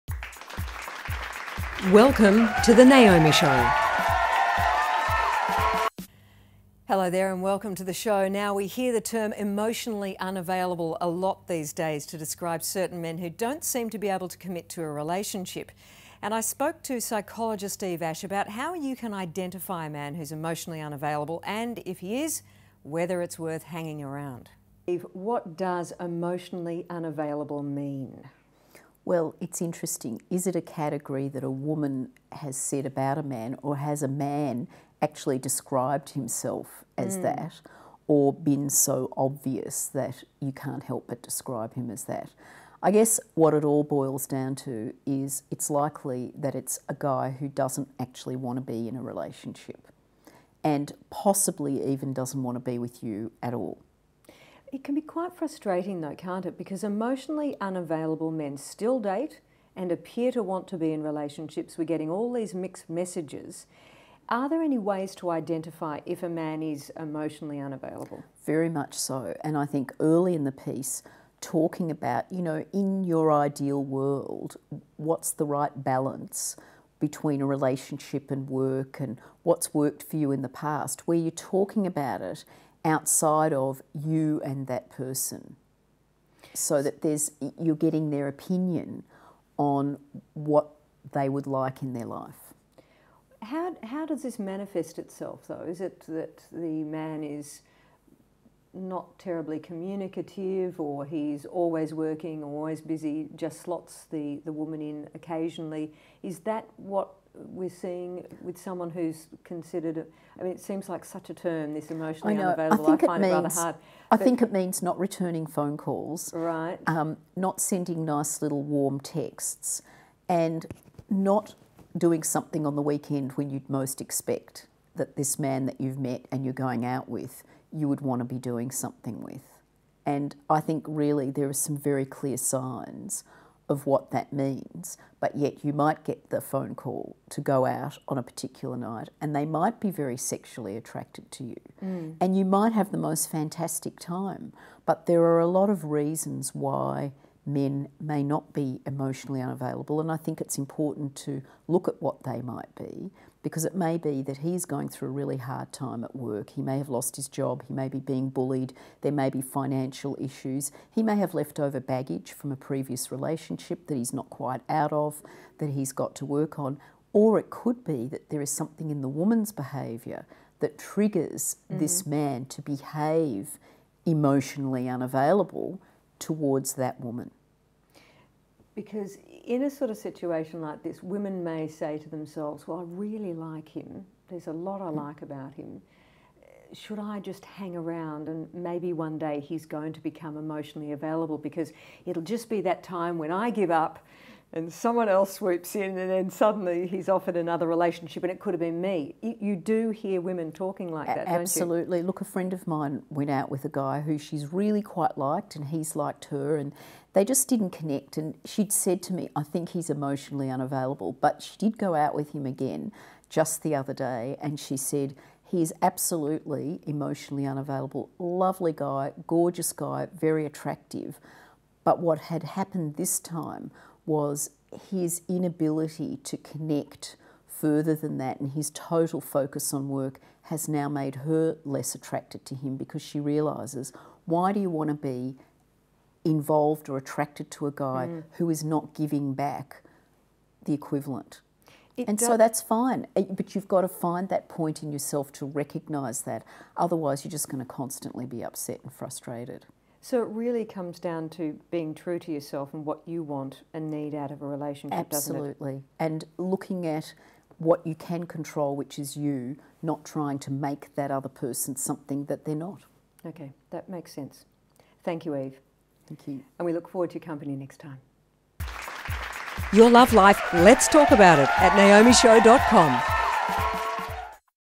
Naomi Robson - How to work out if he's emotionally available
How to work out if he's Emotionally Unavailable - and whether you should cut your losses and move on!

We hear the term emotionally unavailable these days to describe certain types of men who dont want to commit to a relationship. And lets face it, if youre looking for something long term and they dont want to come to party it can be a waste of your time  no matter how great they are or how good the sex is.

Sometimes, it would seem that being emotionally unavailable is just a really good excuse not to get involved in a relationship but there are times when a man simply is not ready or willing to commit to a relationship.

So, Naomi spoke to psychologist, Eve Ash, about how you can identify a man whos emotionally unavailable and whether its worth hanging around.